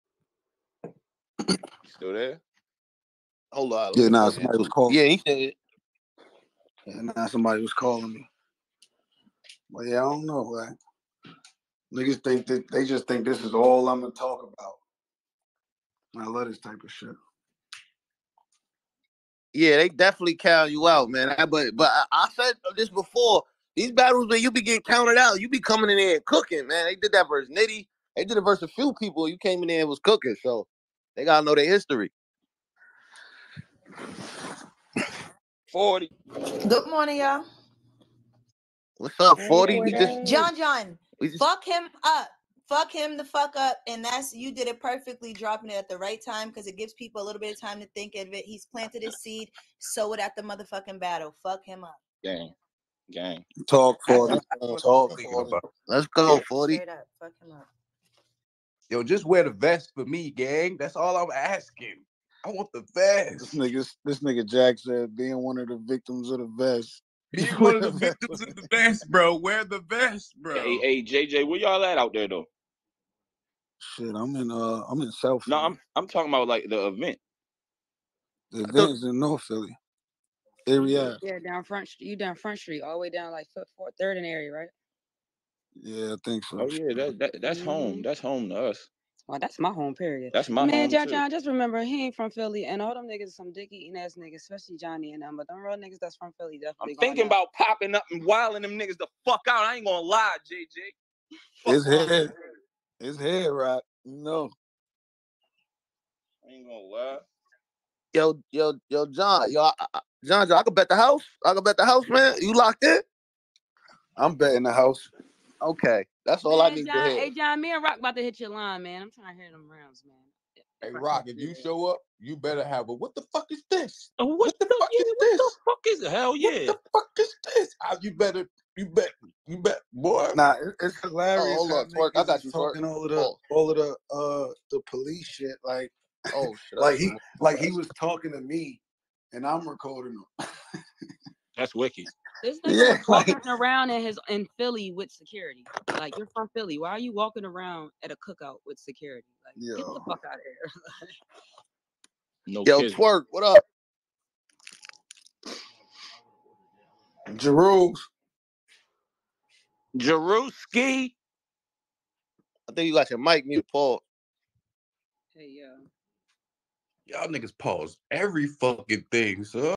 John John the Don ready for Eazy TBC battle
jjd speaks on Eazy the block captain battle